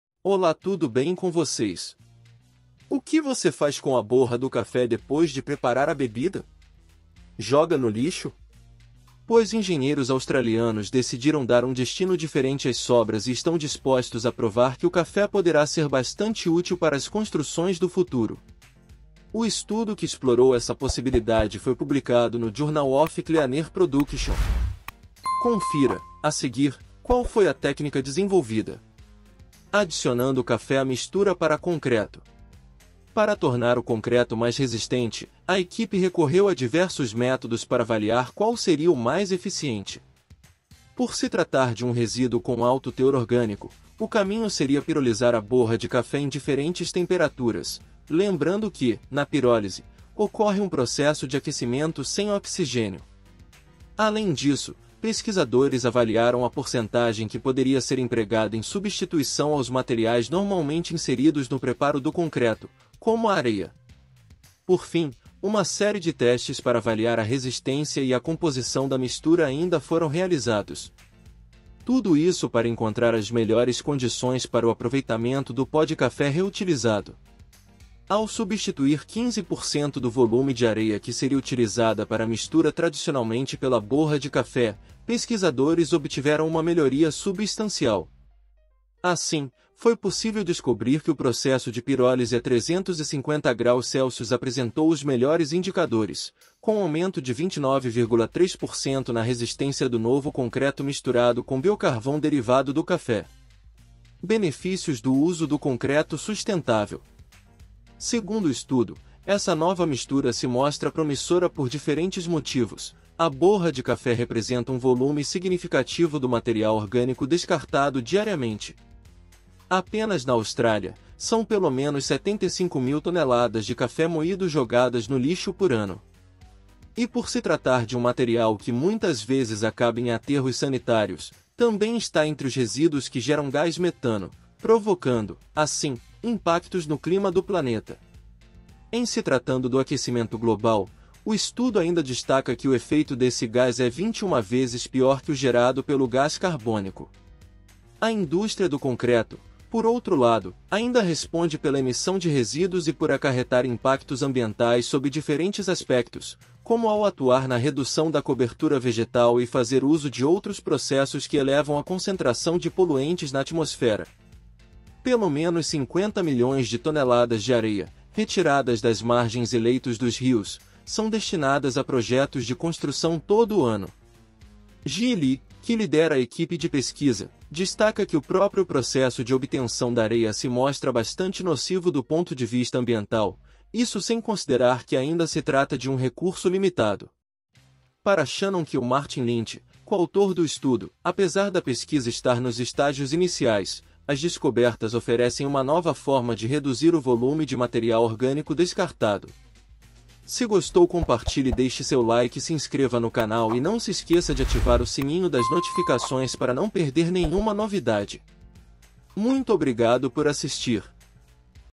🟢 Cientistas usam borra de café para fortalecer concreto de construções
ola tudo bem com voces

O que você faz com a borra do café depois de preparar a bebida? Joga no lixo? Pois engenheiros australianos decidiram dar um destino diferente às sobras e estão dispostos a provar que o café poderá ser bastante útil para as construções do futuro. O estudo que explorou essa possibilidade foi publicado no Journal of Cleaner Production. Confira, a seguir, qual foi a técnica desenvolvida:

Adicionando café à mistura para concreto
Para tornar o concreto mais resistente, a equipe recorreu a diversos métodos para avaliar qual seria o mais eficiente. Por se tratar de um resíduo com alto teor orgânico, o caminho seria pirolisar a borra de café em diferentes temperaturas (lembrando que, na pirólise, ocorre um processo de aquecimento sem oxigênio).

Além disso, pesquisadores avaliaram a porcentagem que poderia ser empregada em substituição aos materiais normalmente inseridos no preparo do concreto, como a areia. Por fim, uma série de testes para avaliar a resistência e a composição da mistura ainda foram realizados. Tudo isso para encontrar as melhores condições para o aproveitamento do pó de café reutilizado.

Ao substituir 15% do volume de areia que seria utilizada para a mistura tradicionalmente pela borra de café, pesquisadores obtiveram uma melhoria substancial. Assim, foi possível descobrir que o processo de pirólise a 350 °C apresentou os melhores indicadores, com aumento de 29,3% na resistência do novo concreto misturado com biocarvão derivado do café.

Benefícios do uso do concreto sustentável
Segundo o estudo, essa nova mistura se mostra promissora por diferentes motivos: a borra de café representa um volume significativo do material orgânico descartado diariamente. Apenas na Austrália, são pelo menos 75 mil toneladas de café moído jogadas no lixo por ano.

E por se tratar de um material que muitas vezes acaba em aterros sanitários, também está entre os resíduos que geram gás metano, provocando, assim, impactos no clima do planeta. Em se tratando do aquecimento global, o estudo ainda destaca que o efeito desse gás é 21 vezes pior que o gerado pelo gás carbônico.

A indústria do concreto, por outro lado, ainda responde pela emissão de resíduos e por acarretar impactos ambientais sob diferentes aspectos, como ao atuar na redução da cobertura vegetal e fazer uso de outros processos que elevam a concentração de poluentes na atmosfera.

Pelo menos 50 milhões de toneladas de areia, retiradas das margens e leitos dos rios, são destinadas a projetos de construção todo ano. Jie Li, que lidera a equipe de pesquisa, destaca que o próprio processo de obtenção da areia se mostra bastante nocivo do ponto de vista ambiental, isso sem considerar que ainda se trata de um recurso limitado.

Para Shannon Kilmartin-Lynch, co-autor do estudo, apesar da pesquisa estar nos estágios iniciais, as descobertas oferecem uma nova forma de reduzir o volume de material orgânico descartado.